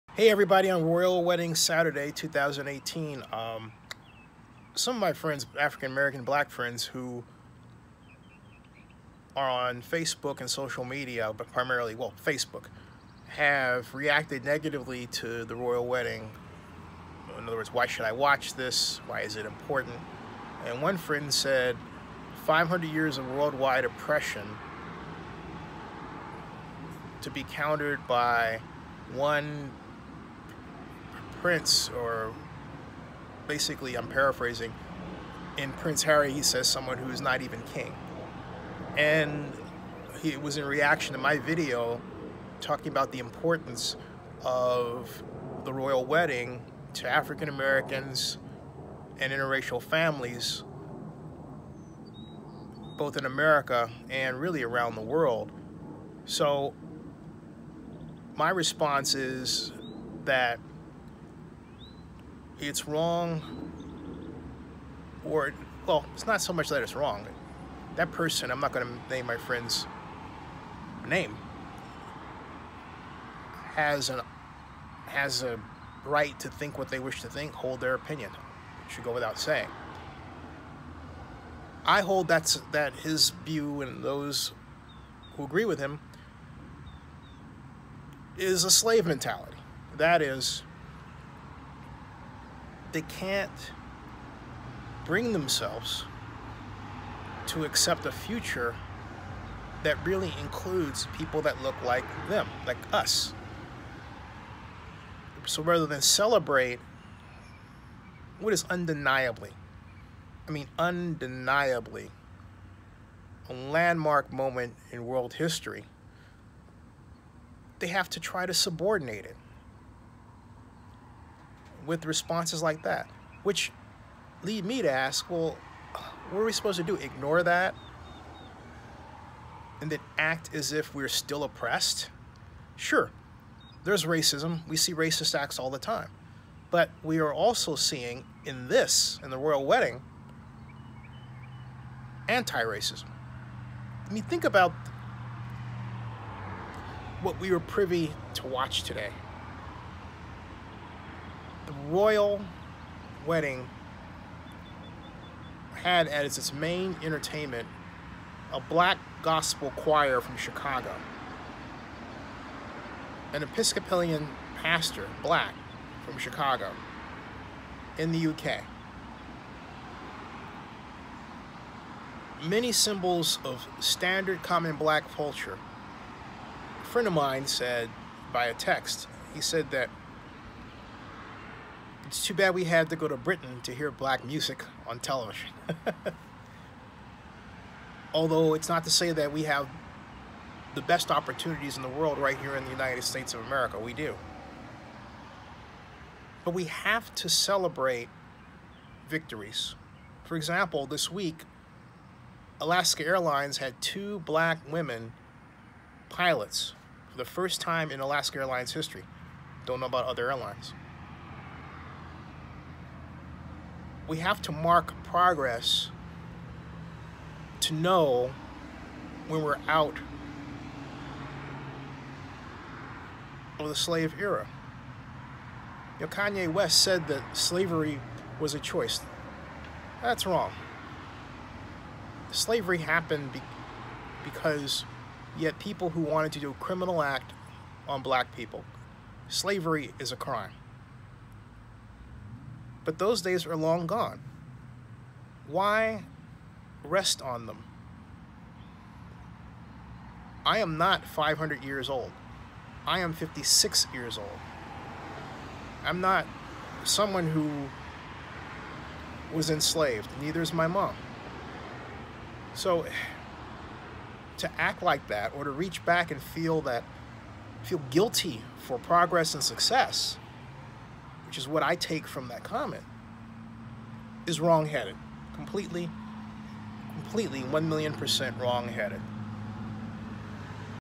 Reacting To The Royal Wedding 2018 With A Modern Slave Mentally Is Wrongheaded
Zennie62 Reacting To The Royal Wedding 2018 With A Modern Slave Mentally Is Wrongheaded

Some of my folks, black friends, have expressed a rather sad reaction to the 2018 Royal Wedding: one friend posted this: “500 years of worldwide oppression being countered by a wedding with someone down the food chain to be King...yeah that answers it all”. And in response to my video explaining why the 2018 Royal Wedding is positive history for African Americans and for interracial families. What he expressed was a slave mentally : reaching back to the past as a reason to ignore the images of progress, today.  I wonder if my friend has a hard time celebrating other blacks who have made it?  I don’t.  In order to not be left behind the waves of progress, we have to not fear to swim in them. Some of us are very afraid to do so. Maria Ayerdi Kaplan